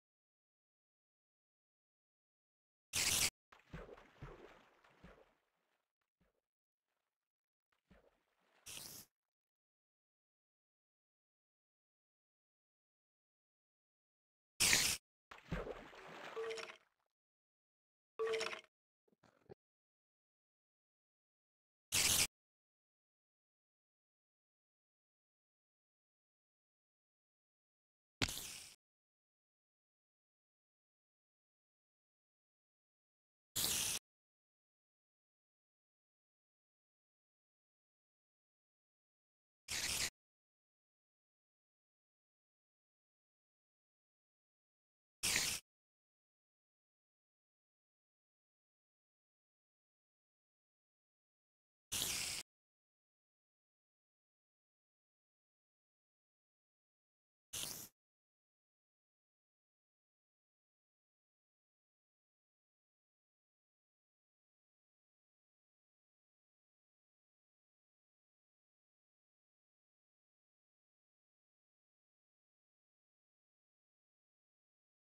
Spider trying out minecraft rail
A spider trying out a unfinished minecraft rail.